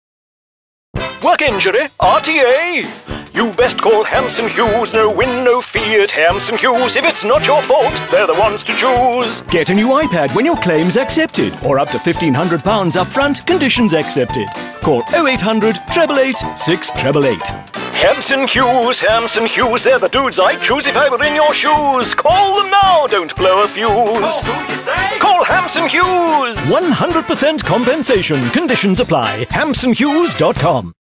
Hampson Hughes Solicitors -- Jazz Hands
If this one didn't get you singing along and swinging your jazz hands from side to side, then there's nothing down for you!
Hampson Hughes Solicitors took our countrywide radio advertising to a jazzy new level in May this year with our new advert, again famously pushing our iPad 2 and cash advance incentives.
Keep your ears peeled for the next instalment in Hampson Hughes Solicitors radio advertising campaign!